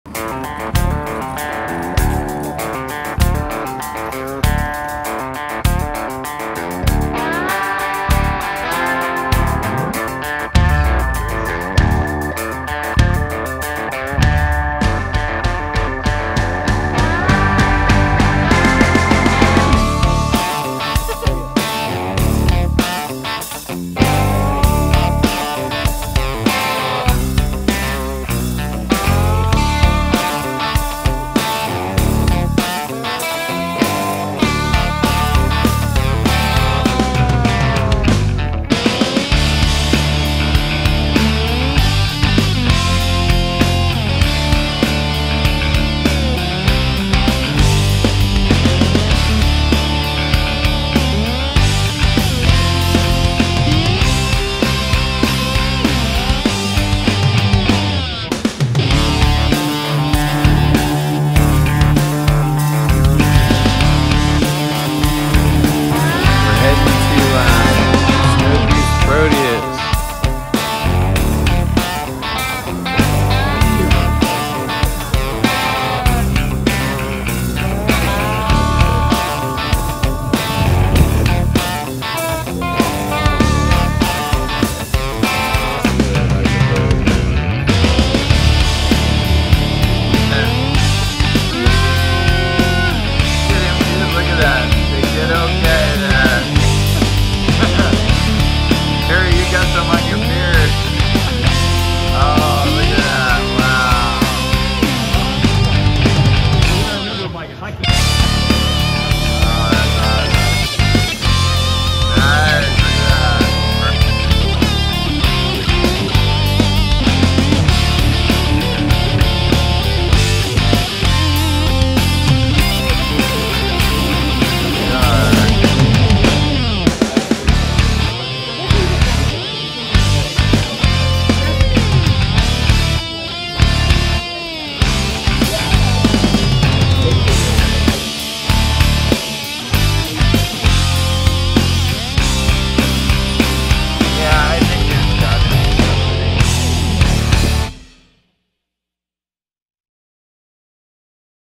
Skagit Valley Tulip Fields - 2023
When the weather in the Cascades is bad (usually in March and April) we take trips to other places, like the Seattle Art Museum or the Skagit Tulip Fields which were so beautiful. Enjoy.